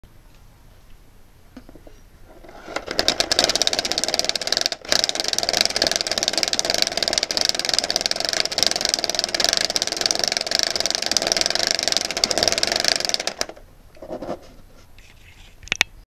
The Calabi-Yau manifold 5-4-tiles Anti-de Sitter Space
The Calabi-Yau manifold is thought to represent the structure of ten-dimensional spacetime in String Theory.   Therefore, the Calabi-Yau manifold must tile spacetime completely.  Here is my proposal for how it might be done.  FlipBooKit Made in Wolfram Language.